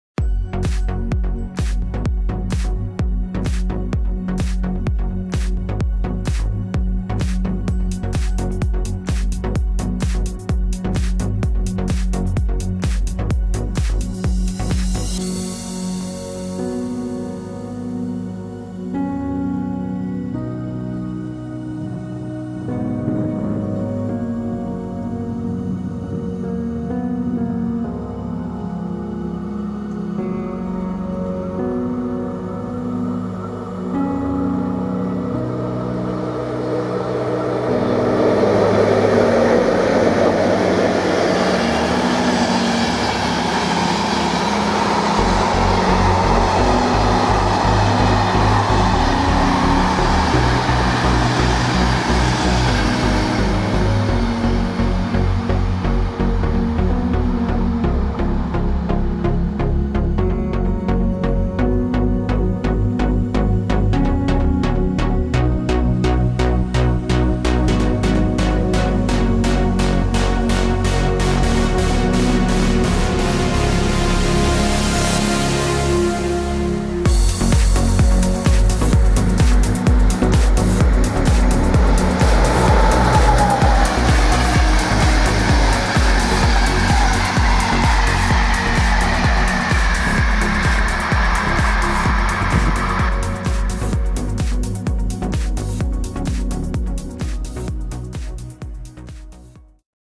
151.006-4 / E 499.2006 - návrat a první nasazení s polepem | viz komentář
Loko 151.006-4 z OCÚ Východ, SÚ Bohumín, byla v DPOV Nymburk opatřena celoplošným propagačním polepem k 25. výročí zřízení Dopravní fakulty Jana Pernera na Univerzitě Pardubice a 25. výročí založení společnosti České dráhy a.s. (autor návrhu proděkan doc. Ing. Radovan Soušek, PhD.). Loko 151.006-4 "absolvovala" vyvazovací opravu v DPOV Přerov a měla být do konce opravy opatřena novým lakem. Bohužel se tak nestalo a byla zařazena do provozu. Dalším "adeptem" na propagační lak se měla stát loko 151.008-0, která prochází vyvazovací opravou od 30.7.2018. Nakonec se rozhodlo, že bude "propagovat" výročí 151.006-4, a proto byla přepravena do Nymburka, kde obdržela "polep". Ve videu zachycen návrat z Nymburka do Bohumína v Sv 1307 a první výkon v novém "kabátku" vlak 116 - Bohumín - Praha (obrat 145)